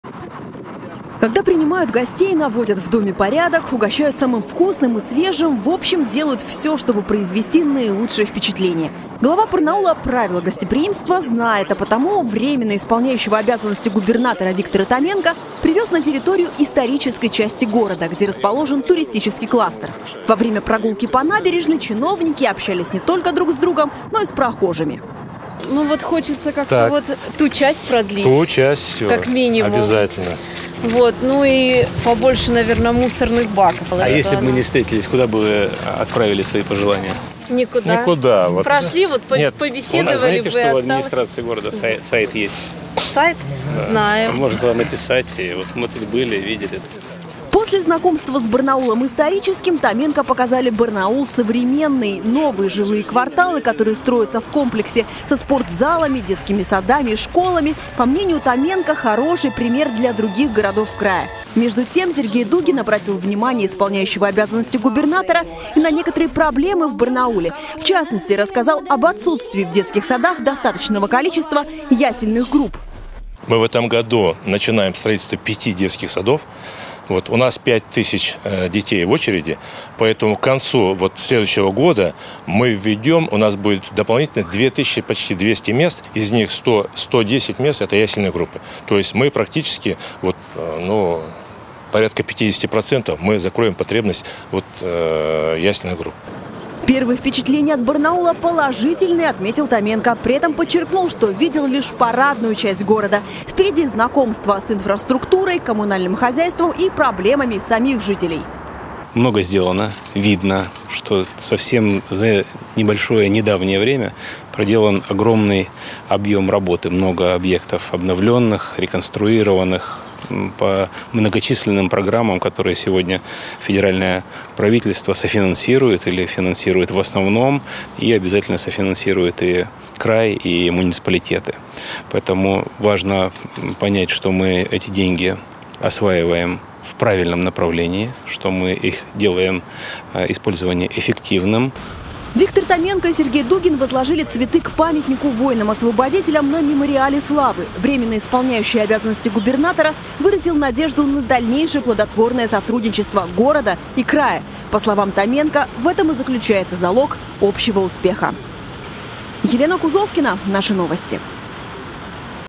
Виктор Томенко остался доволен экскурсией по Барнаулу
«Первые впечатления от Барнаула очень положительные», так сказал  исполняющий обязанности губернатора Виктор Томенко, после воскресной экскурсии, которую для провел для него глава Барнаула Сергей Дугин.